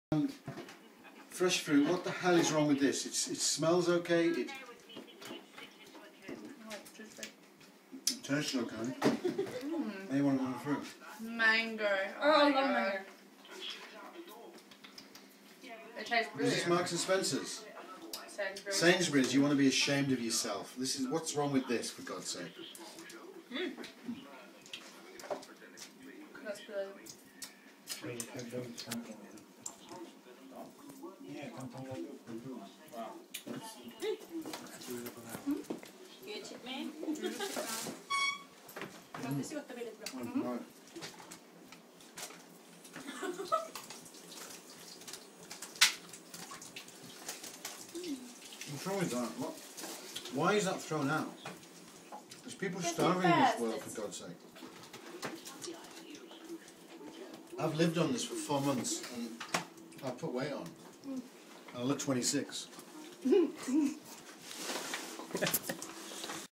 Mark Guard/Belgravia Squatter`s,Enjoy waste food.
Mark Guard and two Belgravia Squatters are eating waste food from Sainsbury`s. Why do the supermarkets have to throw away so mutch unused food. What can be done to correct this ...?